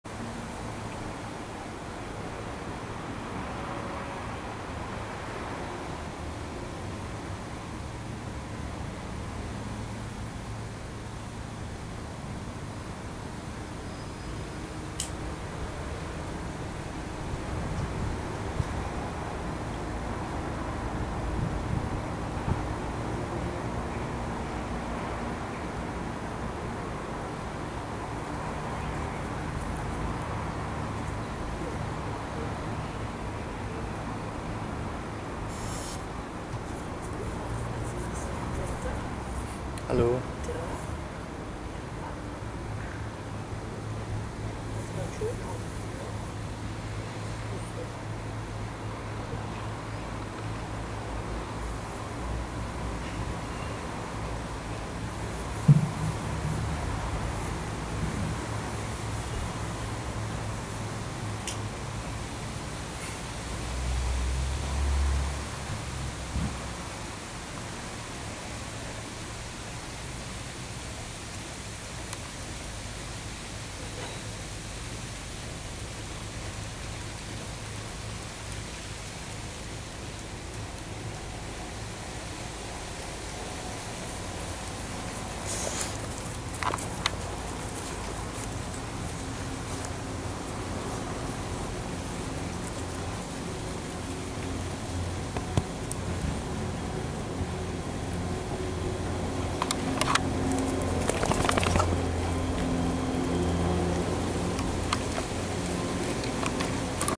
Sony Handycam HDR-CX200E FULL ZOOM
Ich filme mit der Sony HDR-CX200E und nutze dabei den Zoom voll aus.